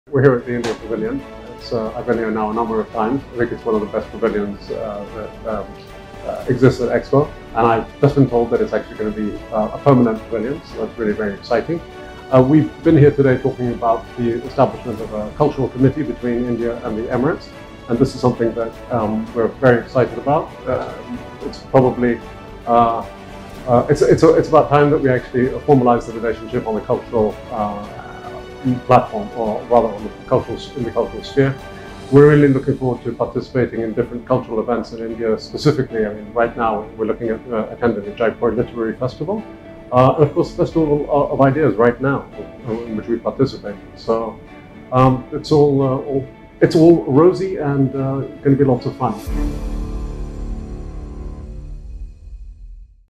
Expo 2020 Dubai | India Pavilion | H.E. Omar Ghobash
I am very excited today to be here and to talk about the cultural unity between India & the Emirates: H.E Omar Ghobash, Assistant Minister for Culture and Public Diplomacy at the UAE Ministry of Foreign Affairs and International Cooperation, UAE and DCG, UAE Pavilion at Expo2020Dubai, during his visit to the IndiaPavilion.